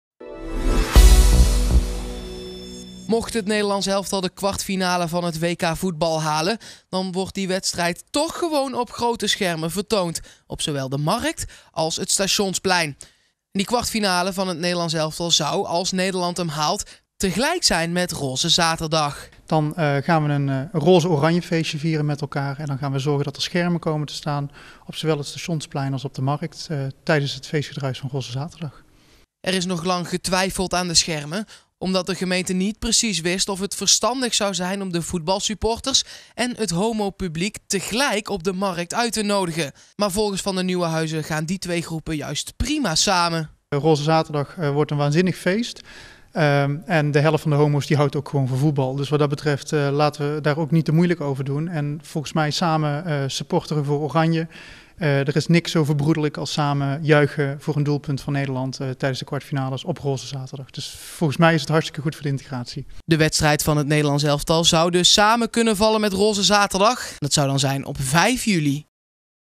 Grote schermen in Eindhoven tijdens WK
Als Oranje de kwartfinale haalt op het WK voetbal, dan komen er grote tv-schermen te staan in de Eindhovense binnenstad, net als vier jaar geleden. Als het zover komt, valt de Oranjedag samen met de festiviteiten rond Roze Zaterdag.